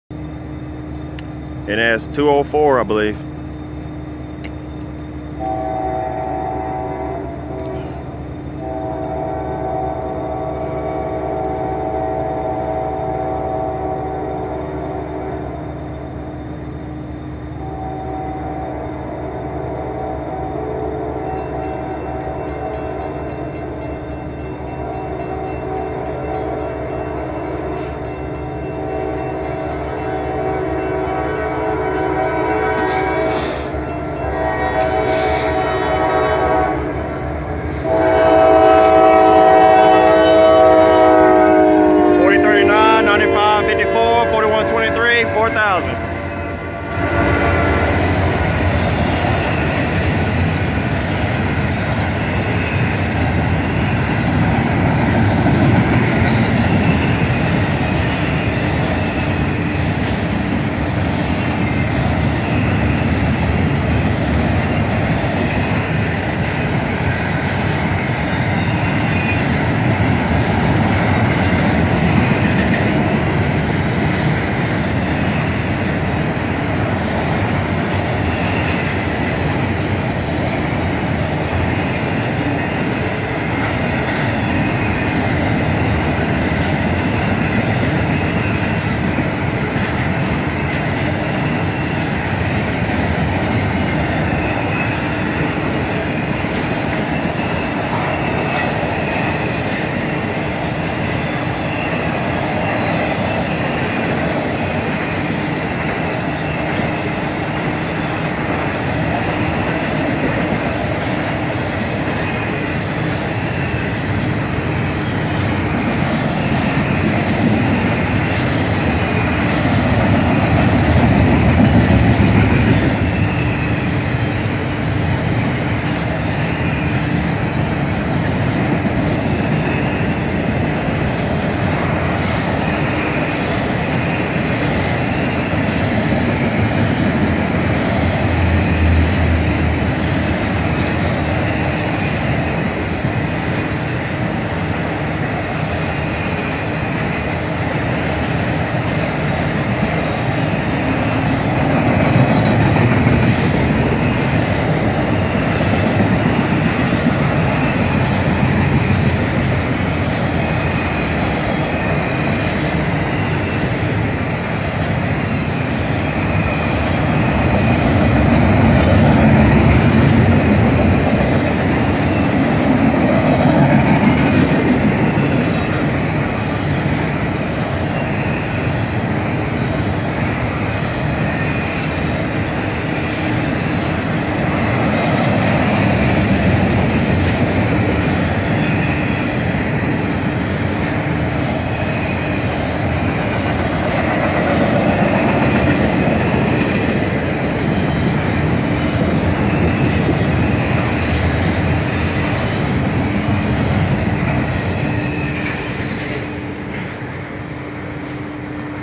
NS 4000 (Sonic Bonnet) trailing on 151-car long 204 thru Gainesville, Ga on 12/24/21
NS AC44C6M 4000 (Sonic Bonnet)